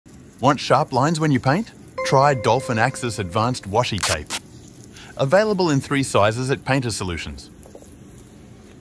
A Painter's Tape that works! Clean Paint lines with Washi Tape.
AXIS is a unique painters tape that utilizes the most Advanced Technology and State-of-the Art manufacturing facility. This new technology uses ultra-flat strong rice paper (washi) with watertight characteristics which allows AXIS to be used for all Interior and Exterior applications.
Featuring proprietary formulation of acrylic based adhesive makes AXIS to be the ideal choice on multi-surfaces, and allows it to stay on surface as long as 60 days, and still can be removed easily without leaving any adhesive residue.